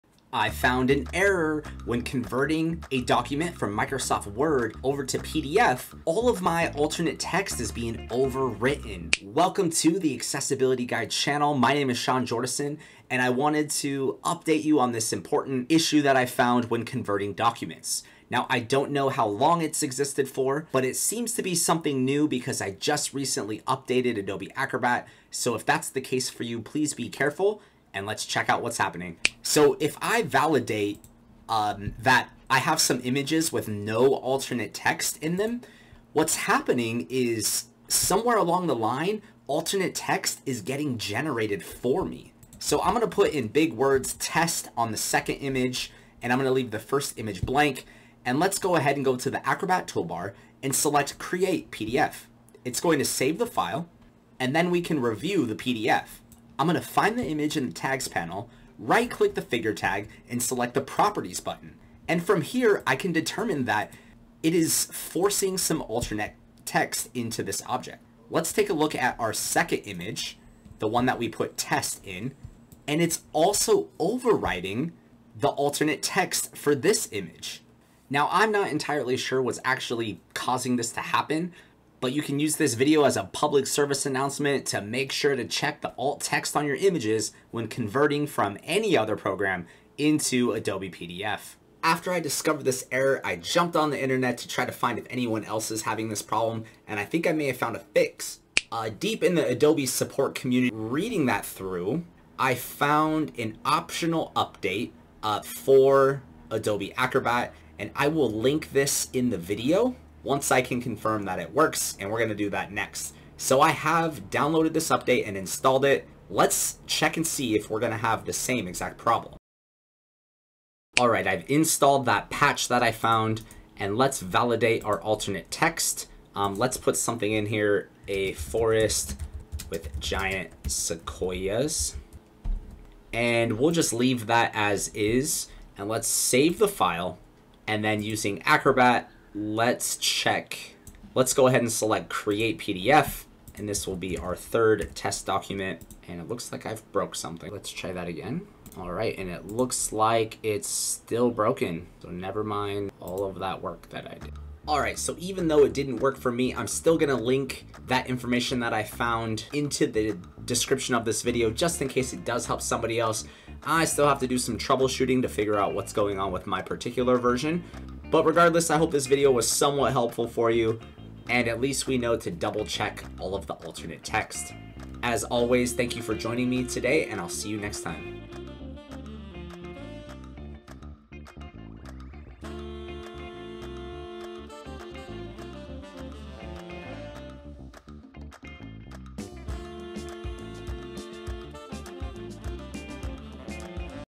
Word to PDF Alternate Text Error | Adobe Acrobat Pro DC
This video will go over a bug that I found when converting a document from Word to PDF. All of my alt text is getting overwritten!